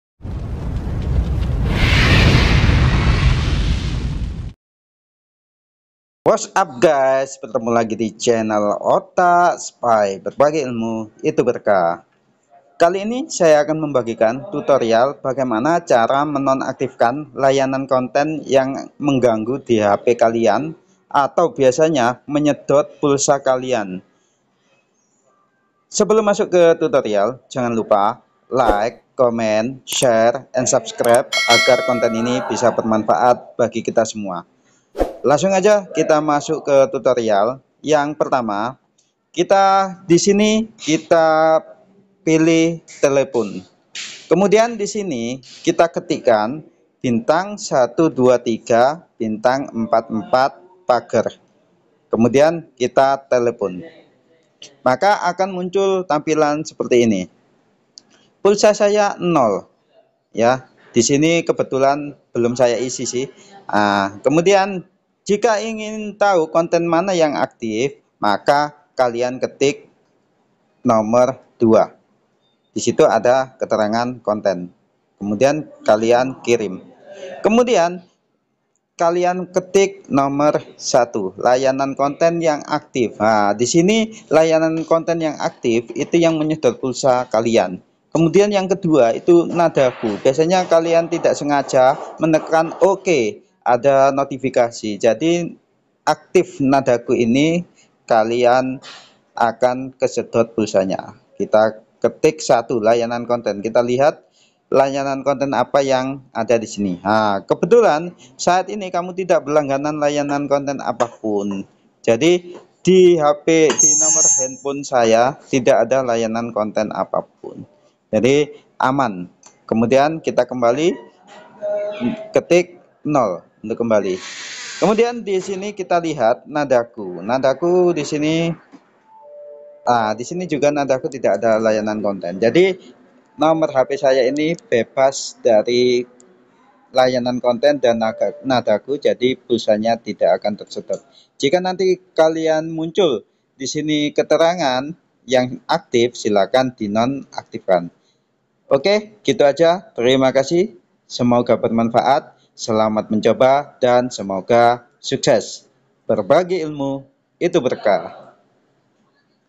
Cara menonaktifkan layanan konten penyedot pulsa Indosat Ooredoo tahun 2024 terbaru di semua HP
video ini sebagai informasi bagi pengguna Indosat agar tidak panik jika tiba-tiba pulsa kita habis karena tersedot layanan konten yang ada di nomor hp kita. ini adalah solusinya

by. otakspy
berbagi ilmu itu berkah

FANSPAGE FACEBOOK : Saiful Prarima Yuda
SUMBER / SOURCE : Microsoft Word